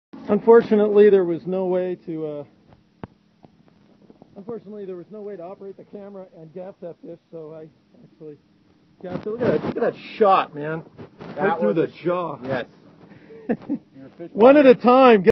Ling2
Part 2 of Kirk and Mikey's lingcod fishing trip for Sea Forager Seafood on August 18th, 2005. It's what's for dinner!